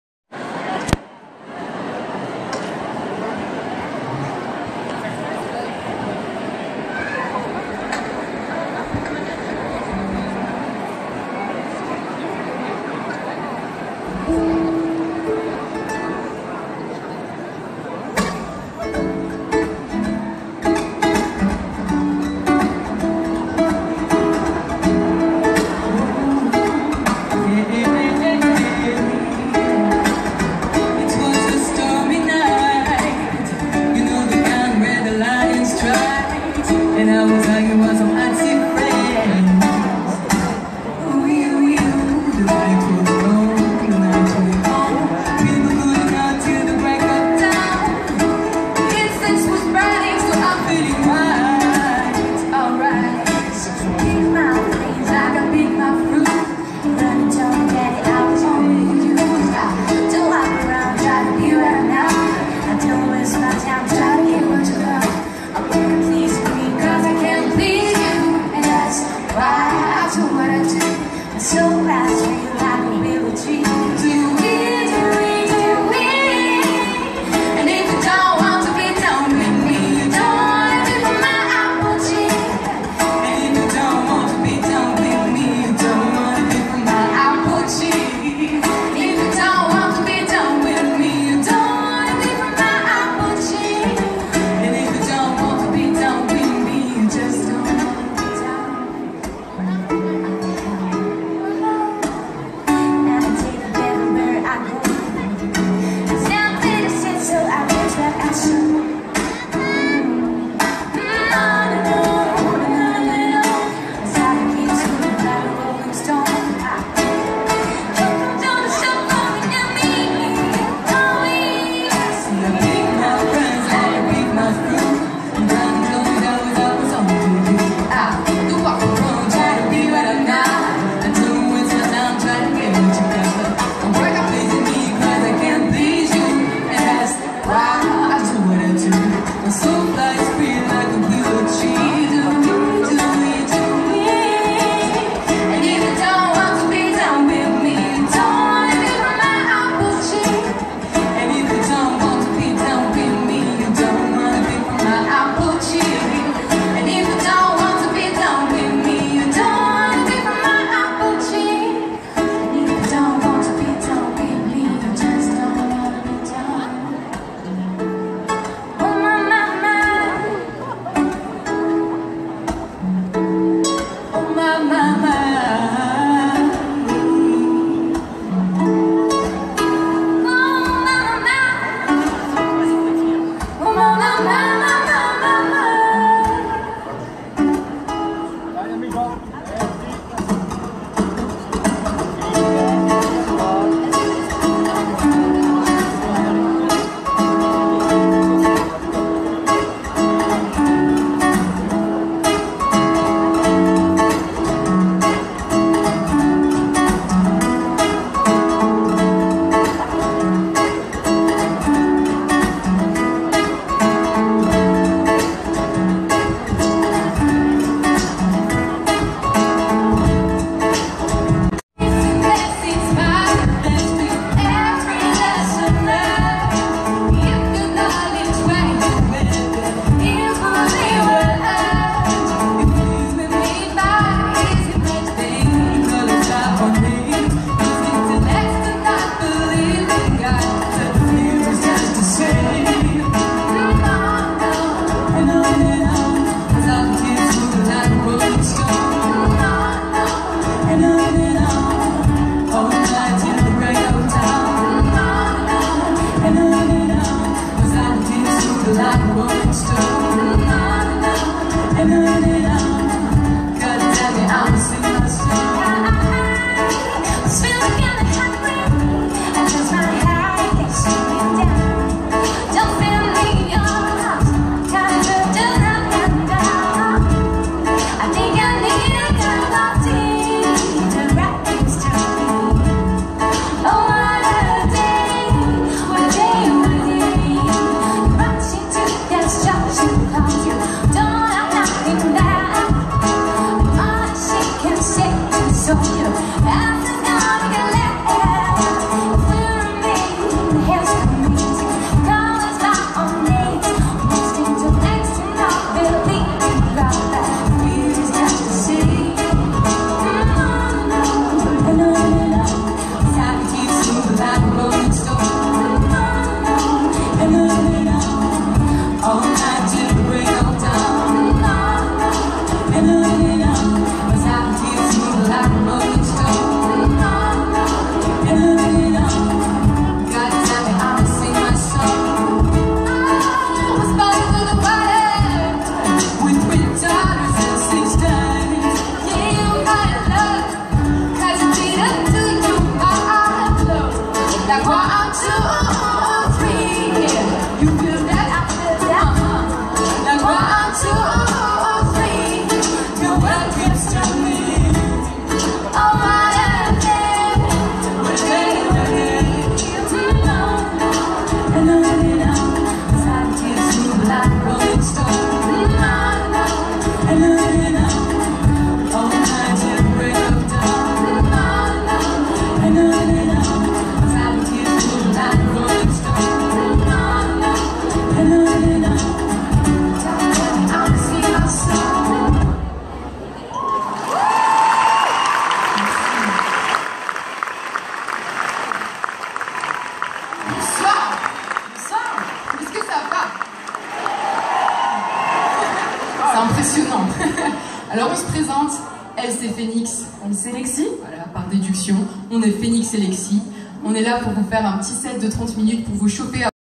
Phénix et Lexy avant LEJ/L.E.J Live, Eden scène de Sausheim, (29/05/2016)
Phénix et Lexy est un Duo Vocal